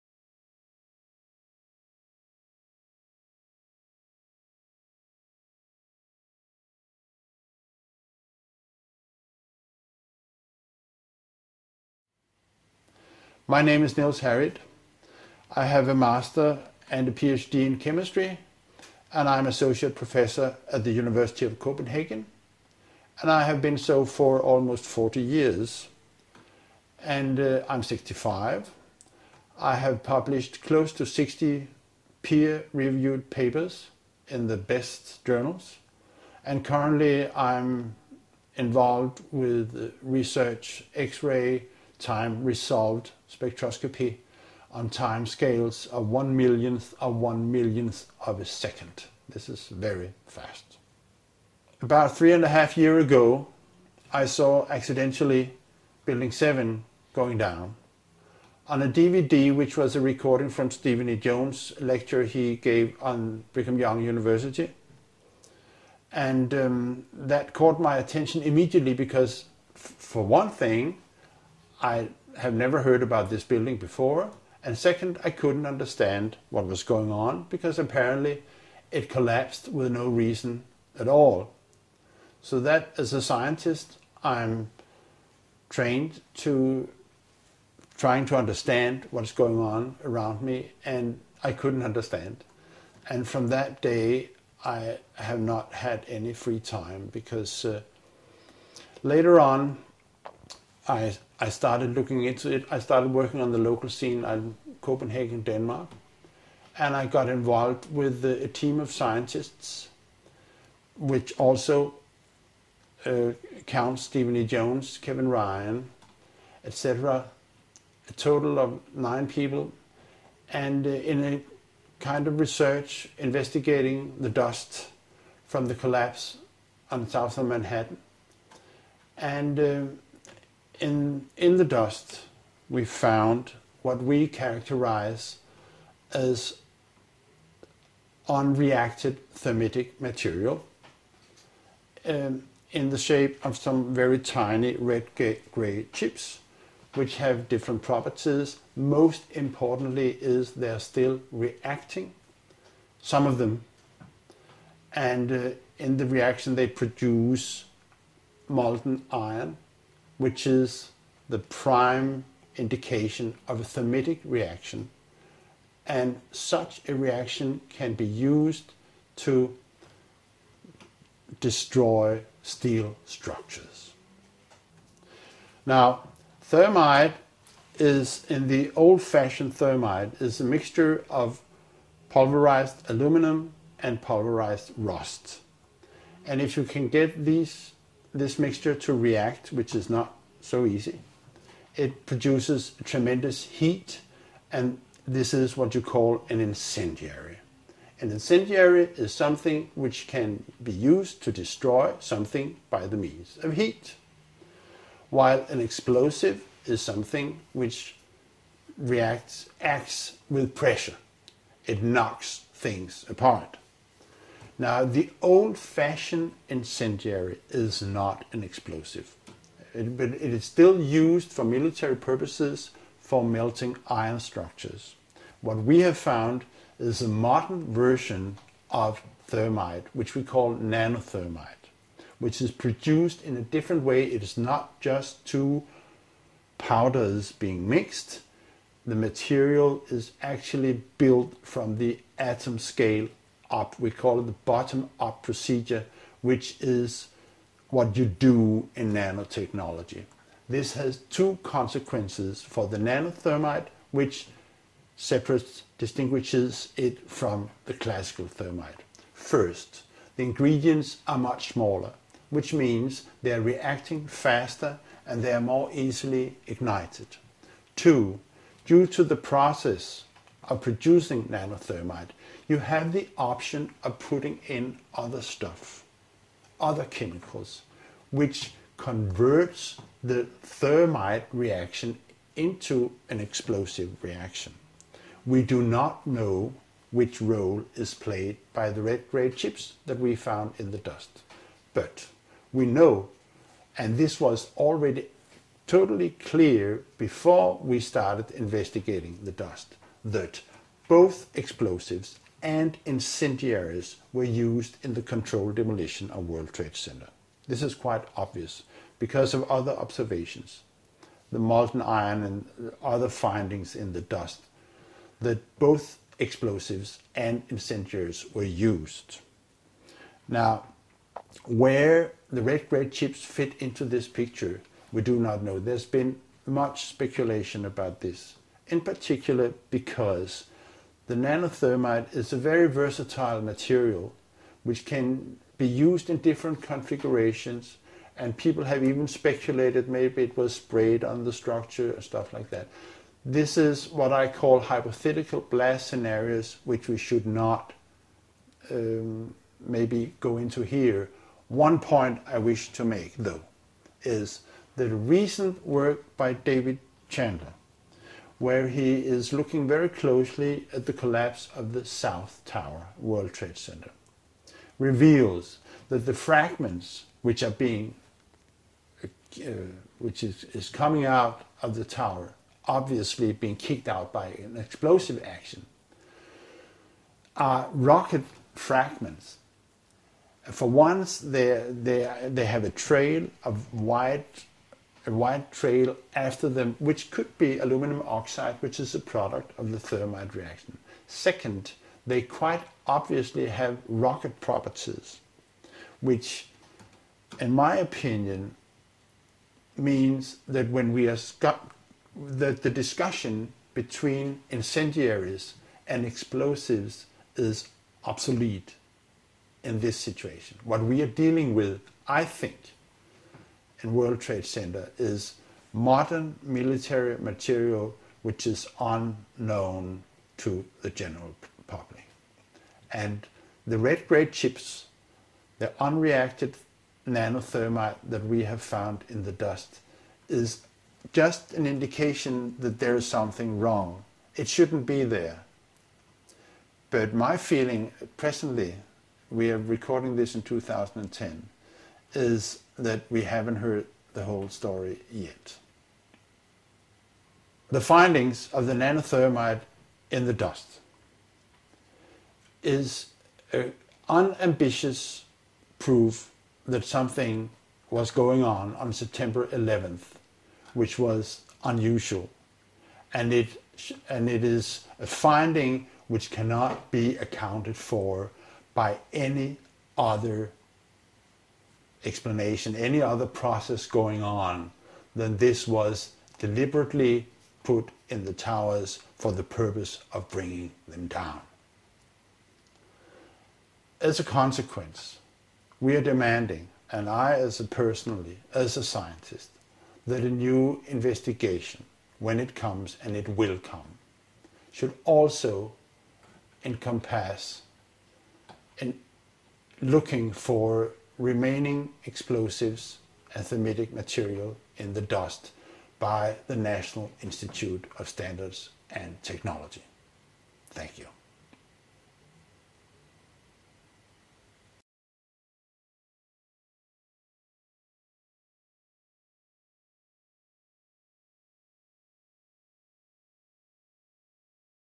Falam os Especialistas - Niels Harrit - legendas em português
ENTREVISTA EXCLUSIVA do AE911Truth!
Químico Niels Harrit - um dos cientistas que encontraram thermite
na poeira do World Trade Center discute Thermite, Nano,termita, Explosivos e Incendiarios.
Essa entrevista é uma filmagem crua de um dos especialistas mundiais que aparecerão no vindouro explosivo documentário do Arquitetos e Engenheiros
"911: Evidência Explosiva - Especialistas falam"
Leia o Open Chemical Physics Journal, 2009, 2, 7-31 -- " Active Thermitic Material Discovered in Dust from the 9/11 World Trade Center Catastrophe " por N.H. & al

Visite a secção em Português da AE911Truth bem como a página do Facebook em Português

Niels Harrit - Químico - Portuguese